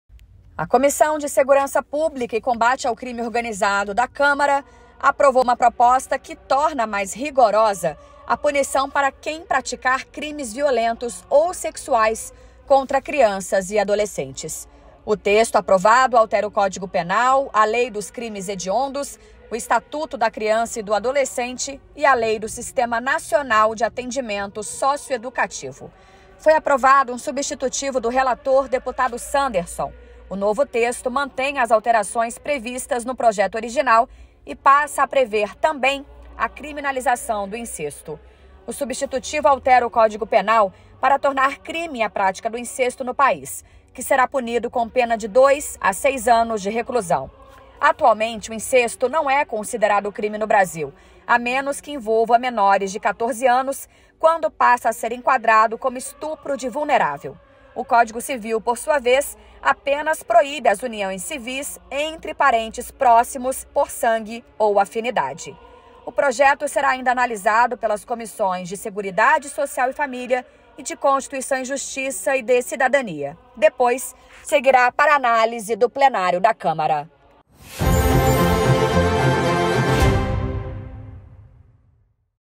Parecer de Sanderson criminaliza incesto e aumenta pena para crimes contra crianças e adolescentes
PL-TV: Parecer de Sanderson criminaliza incesto e aumenta pena para crimes contra crianças e adolescentes